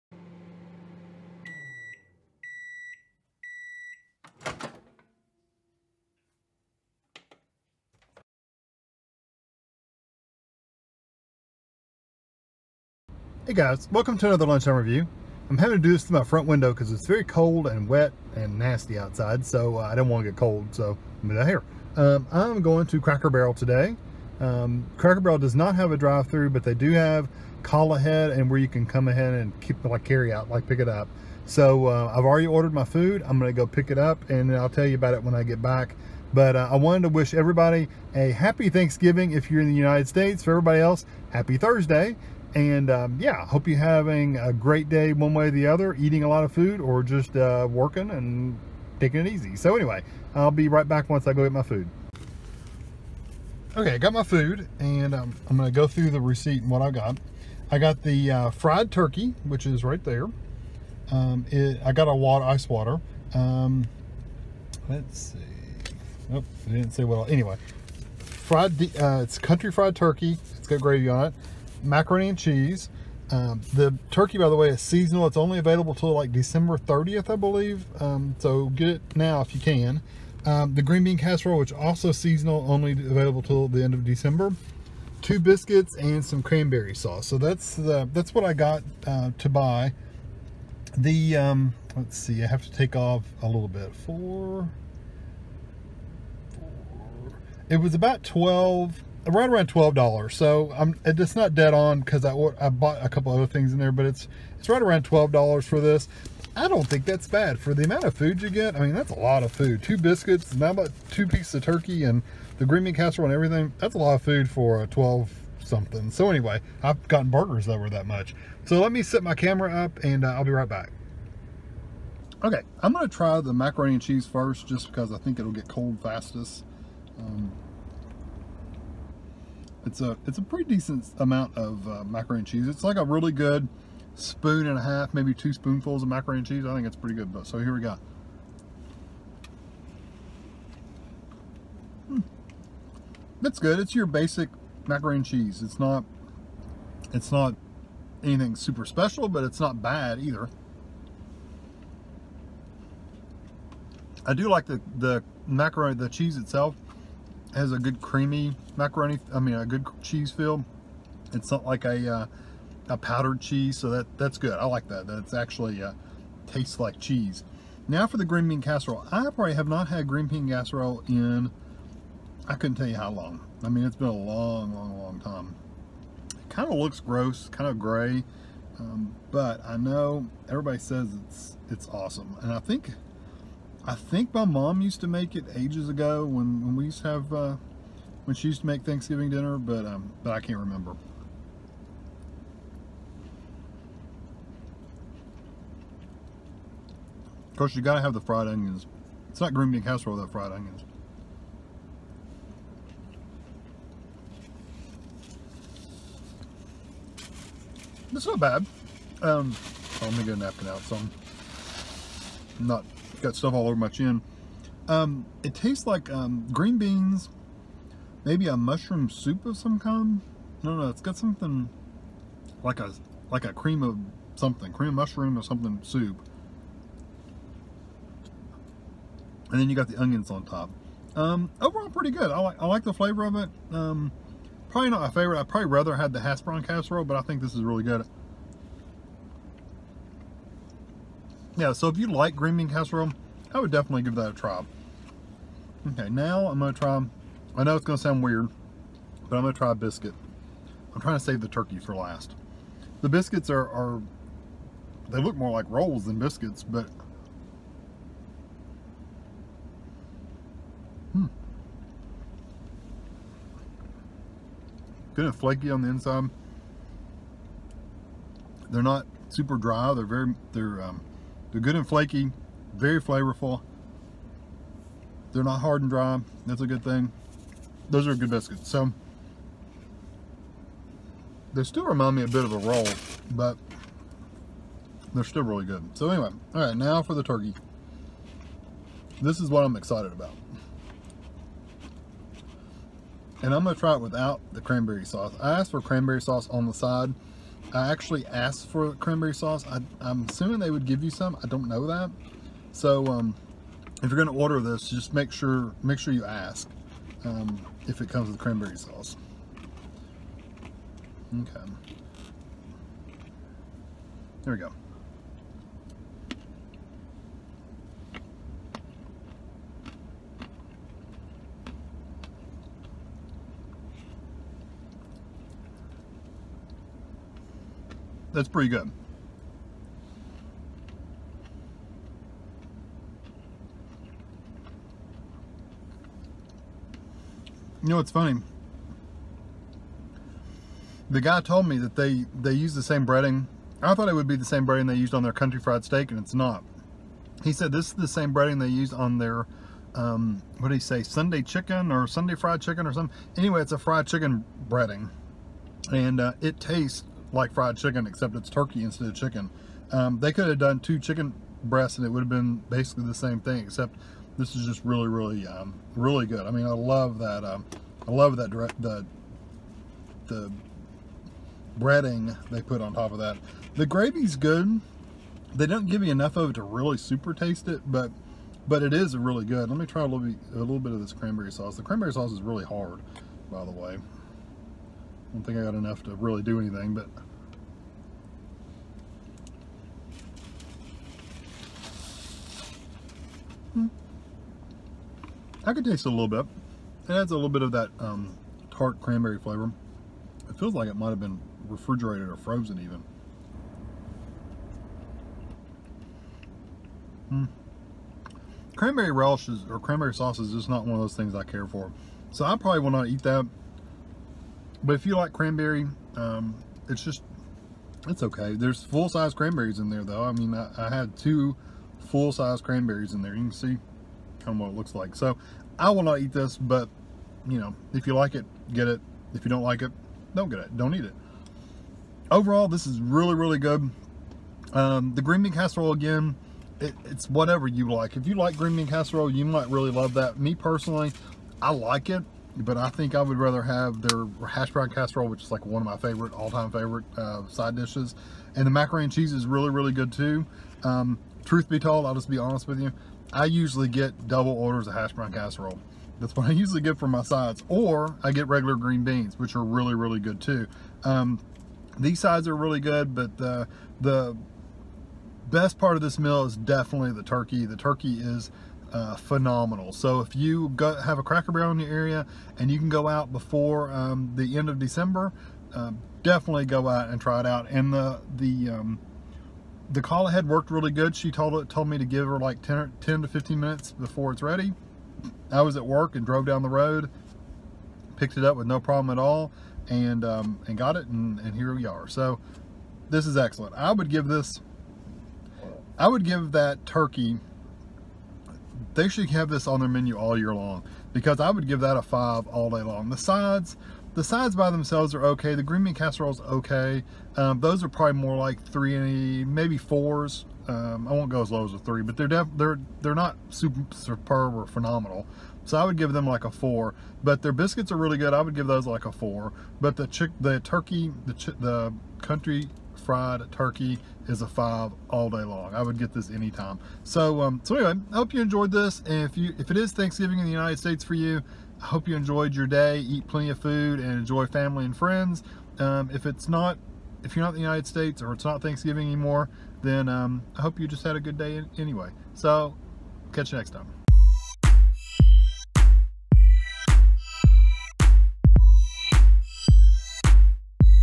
Cracker Barrel Thanksgiving Dinner: Country Fried Turkey, Green Bean Casserole, Cranberry Sauce
In this video, I am reviewing the seasonal Country Fried Turkey, seasonal Green Bean Casserole, Macaroni & Cheese, Cranberry Sauce and Biscuits.